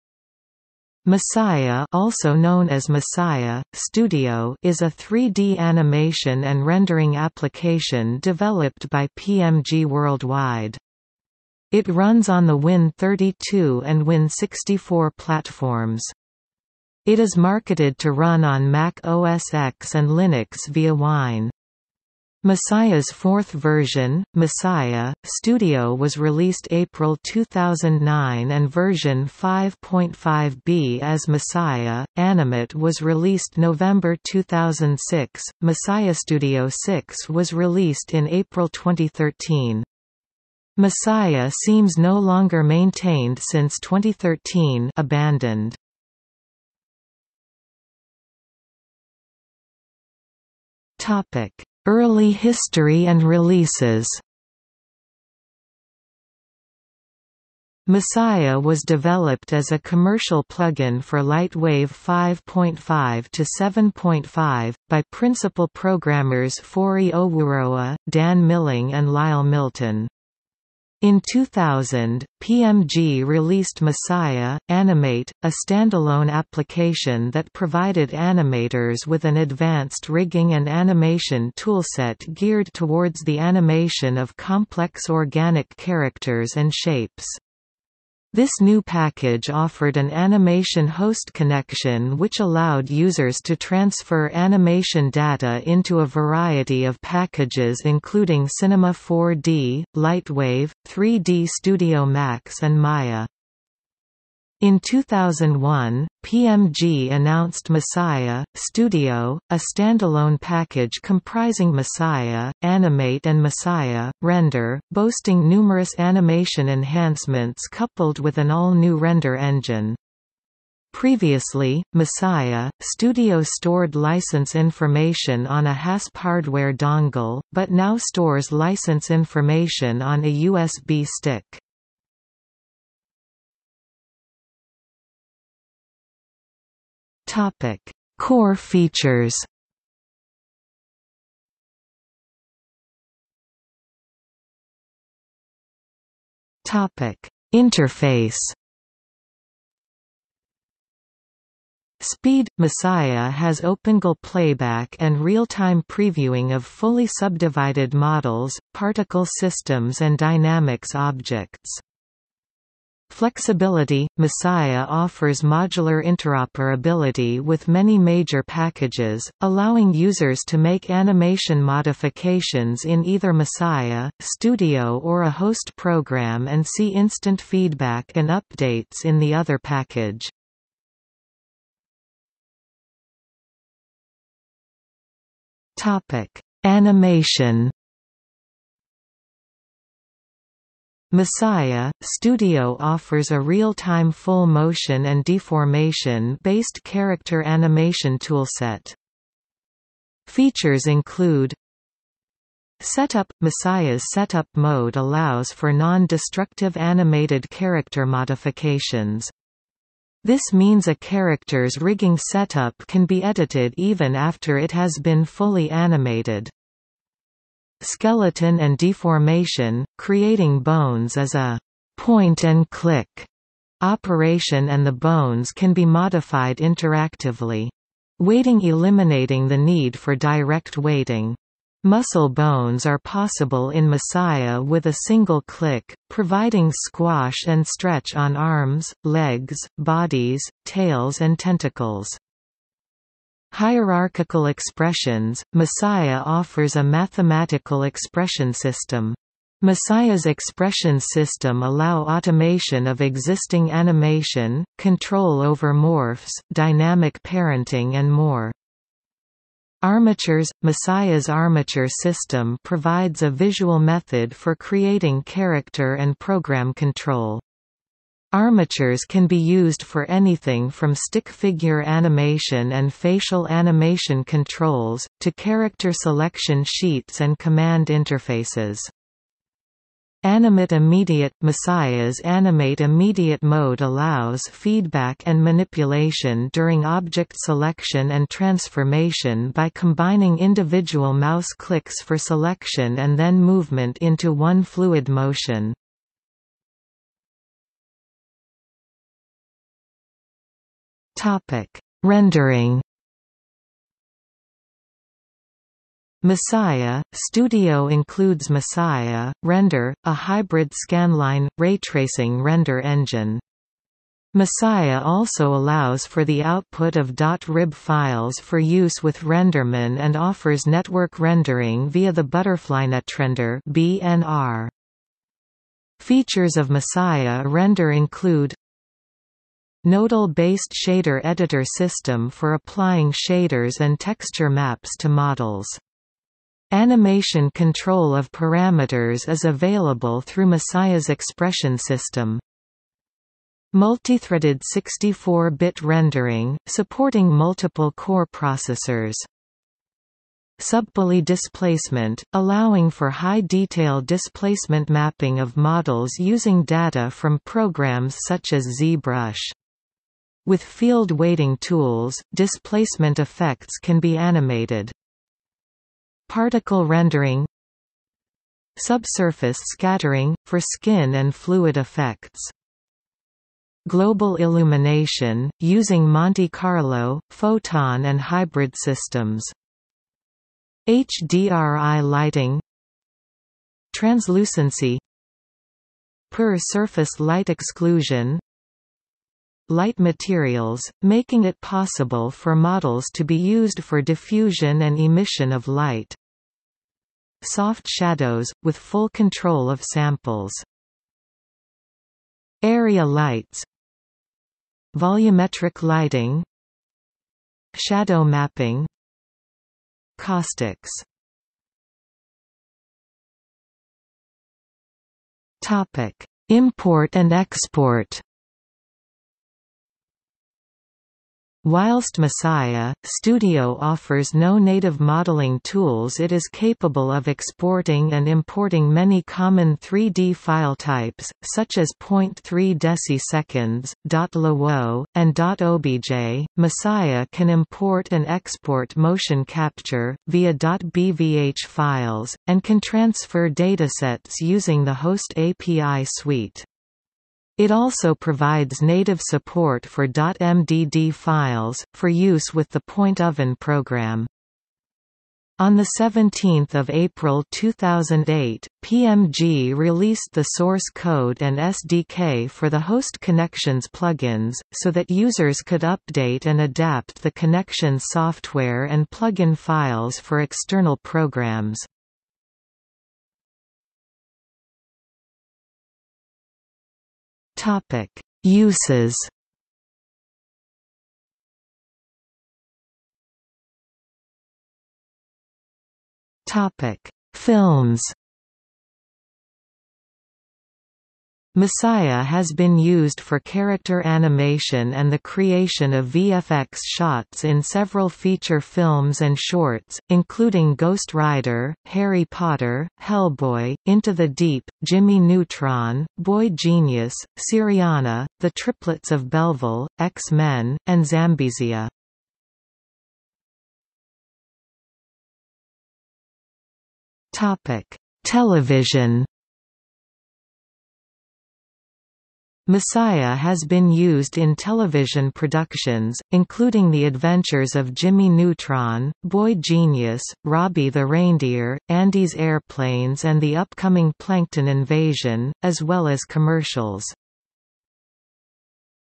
Messiah (software) | Wikipedia audio article
00:00:47 1 Early history and releases
00:02:13 2 Core features
00:02:23 2.1 Interface
00:03:02 2.2 Animation
00:04:57 2.3 Rendering
00:06:52 2.4 Import and export
00:07:54 3 Uses
00:08:03 3.1 Films
00:08:34 3.2 Television

- Socrates

SUMMARY
Messiah (also known as messiah:studio) is a 3D animation and rendering application developed by pmG Worldwide. It runs on the Win32 and Win64 platforms. It is marketed to run on Mac OS X and Linux via Wine. Messiah's fourth version, messiah:studio was released April 2009 and version 5.5b as messiah:animate was released November 2006. messiahStudio6 was released in April 2013. Messiah seems no longer maintained since 2013 (abandoned).